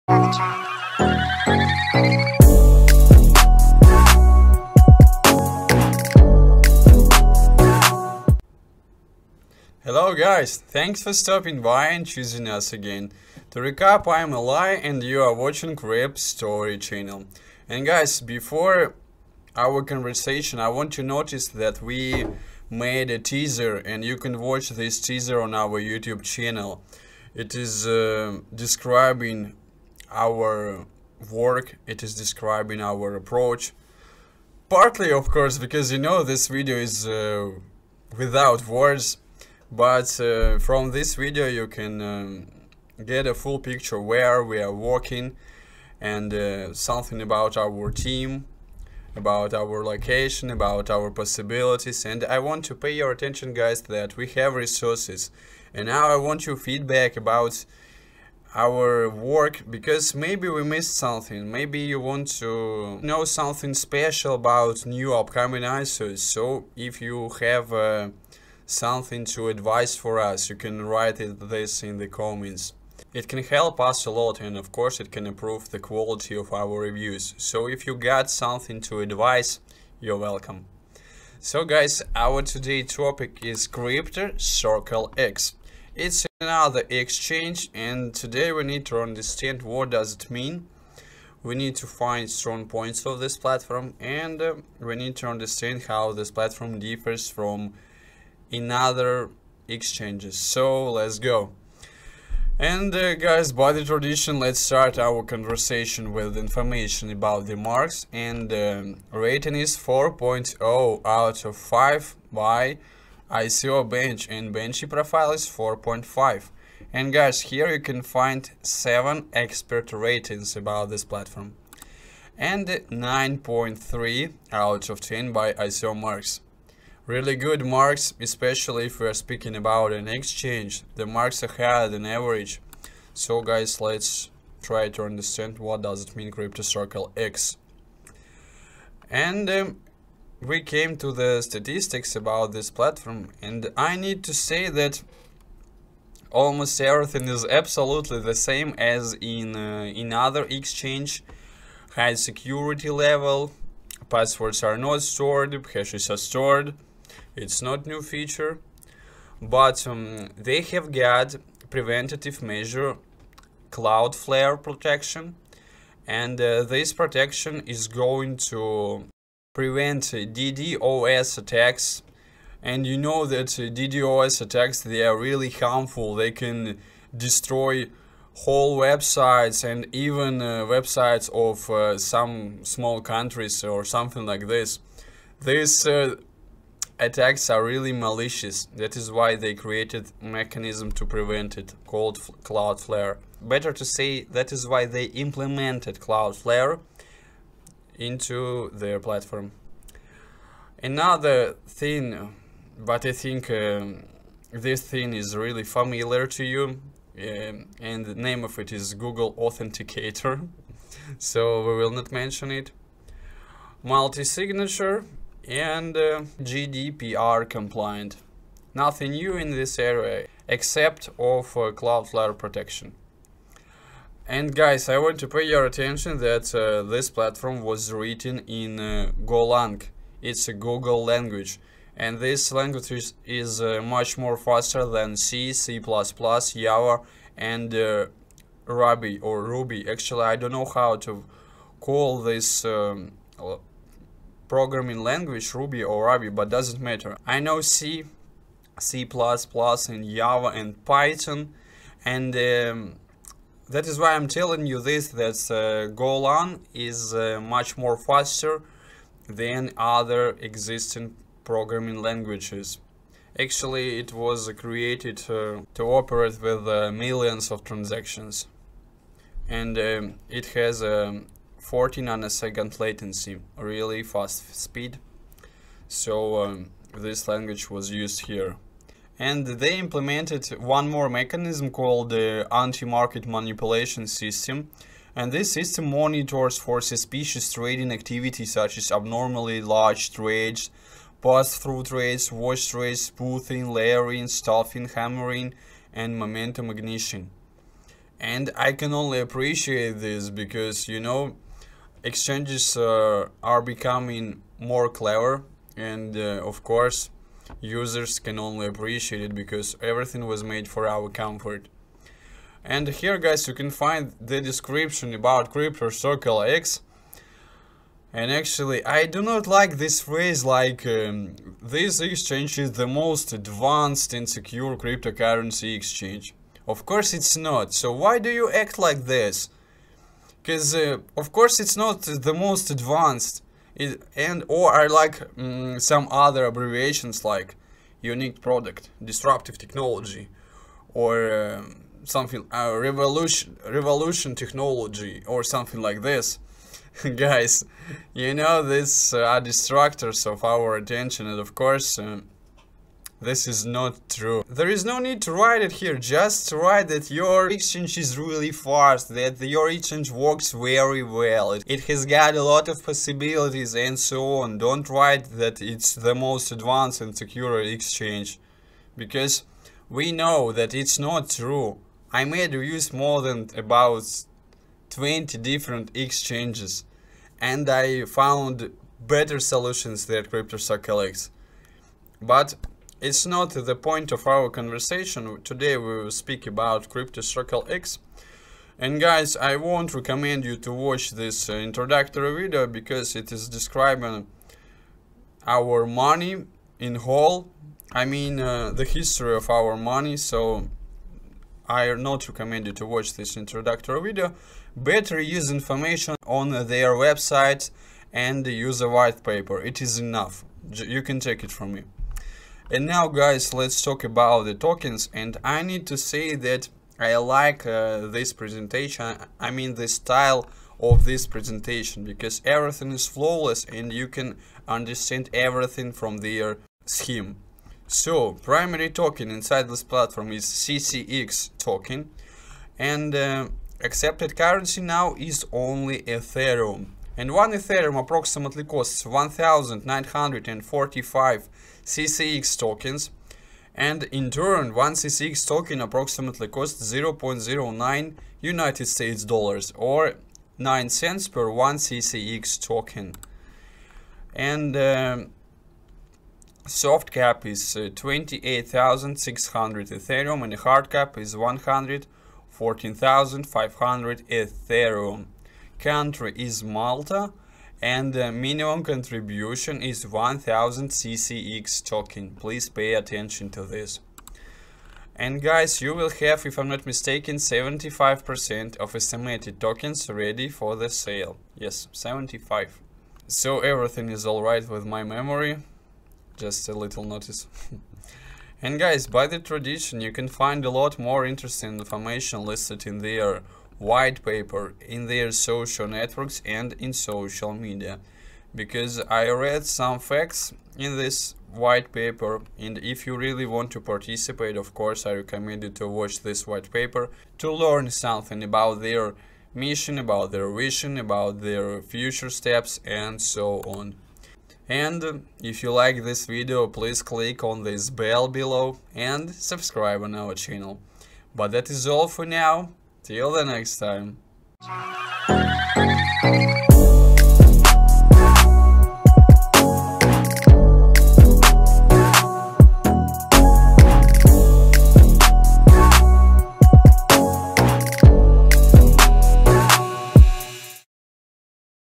Crypto Circle X Creates a Blockchain Infrastructure for Traders
00:00-inception
01:45-ratings
02:17-description and features
06:53-tokens

Crypto Circle eXchange is powered by a superior technological algorithm that is capable of high performance trading with over 10 million transactions per second, A.I. bot, copy trading, auto-trading, professional charts with technical analysis, trading alerts, and much more, in a user friendly and responsive interface or in our specially designed mobile app.